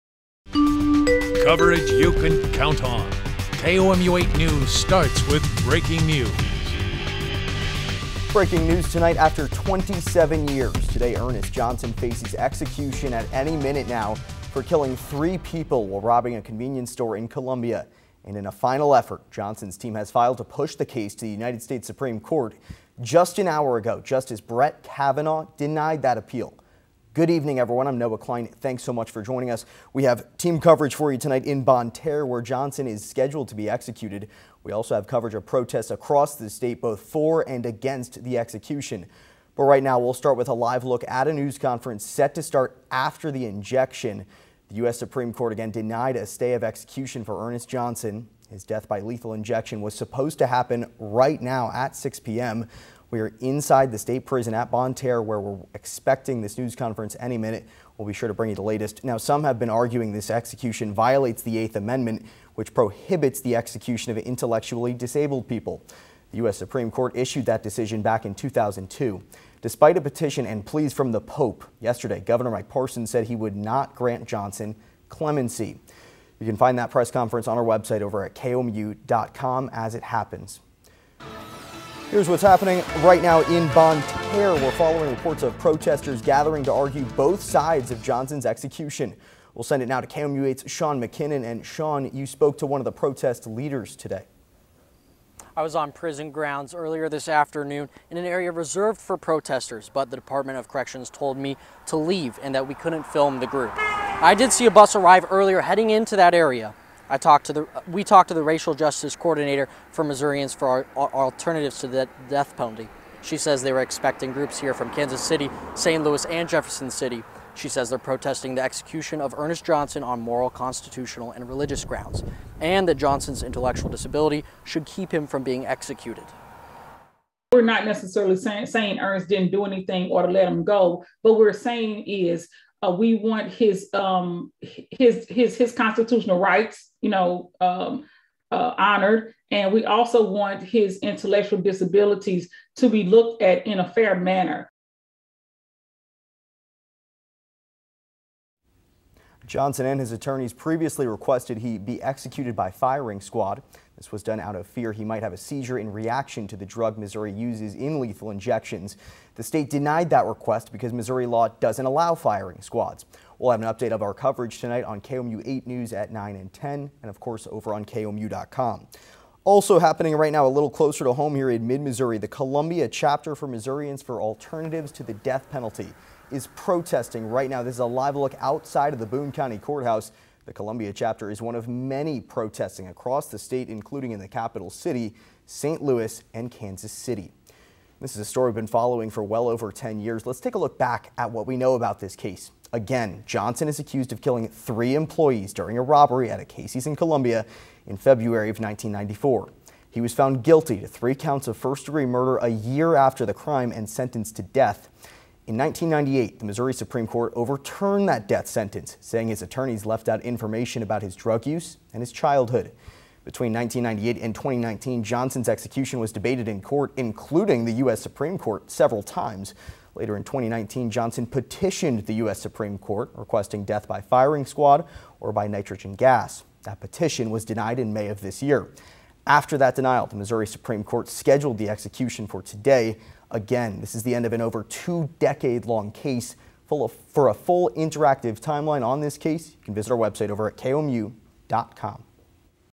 State executes Ernest Johnson 27 years after Columbia triple murder
Johnson was convicted of killing three convenience store workers in Columbia during a robbery in 1994.